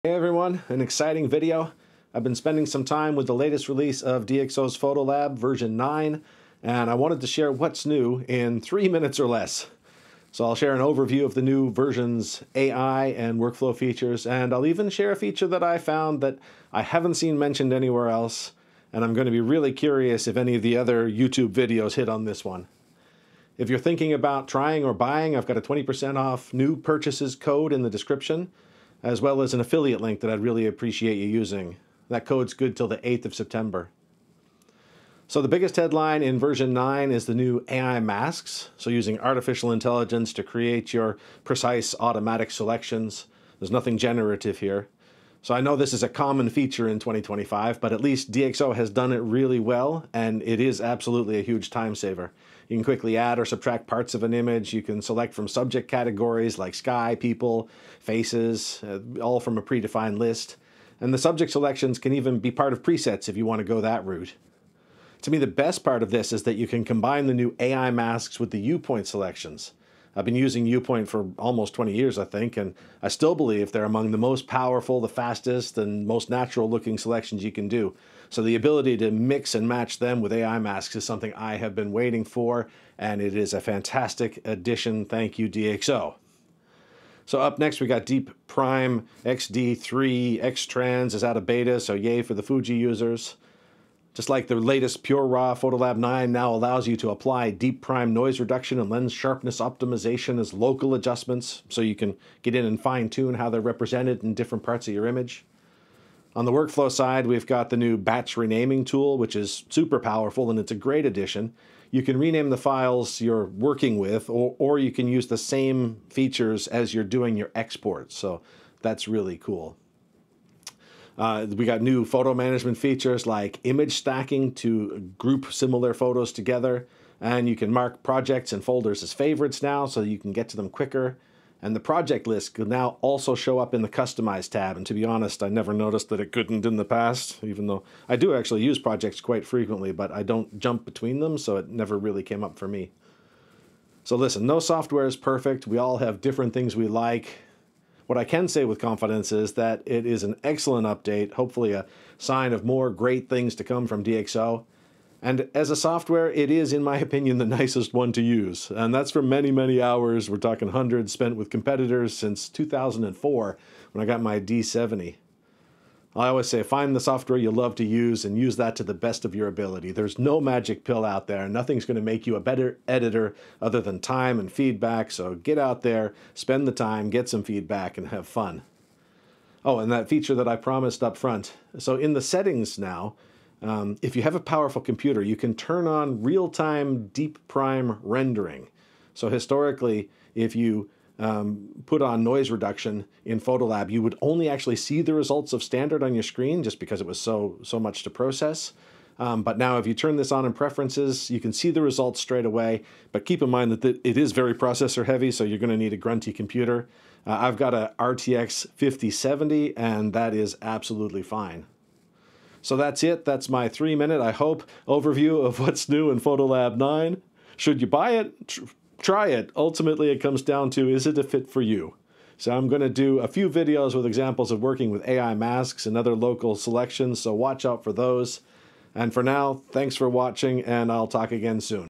What's New in DxO PhotoLab 9 (in Under 4 Minutes)
I've been testing the latest release of DxO PhotoLab 9, and in this video, I'm sharing all the most important new features in four minutes or less! We'll cover the game-changing AI Masks, huge update for Fujifilm users, powerful workflow improvements, and even a "hidden" feature I haven't seen mentioned anywhere else.

DISCOVER MORE PHOTOLAB 9 TIPS
I'm creating a whole series of videos on the new features. Check out the full playlist for more tutorials!

In this quick overview, we cover:

✅ **New AI Masks:** A huge time-saver for making precise selections. I'll explain how you can combine them with the classic U-Point technology for the best of both worlds.

✅ **DeepPRIME XD3 for Fuji:** Great news for X-Trans sensor users! The best noise reduction is now available for you.

✅ **Local DeepPRIME & Lens Sharpness:** You can now apply DxO's incredible noise reduction and lens corrections as a local adjustment for fine-tuned control.

✅ **Workflow Upgrades:** A look at the powerful new Batch Renaming tool, plus new photo management features like Image Stacking and Favorite folders.

✅ **The "Hidden" Feature:** Discover the new preference setting that allows for real-time DeepPRIME rendering if you have a powerful enough computer!

Is DxO PhotoLab 9 worth the upgrade? Watch the video for my quick take, and try it for yourself.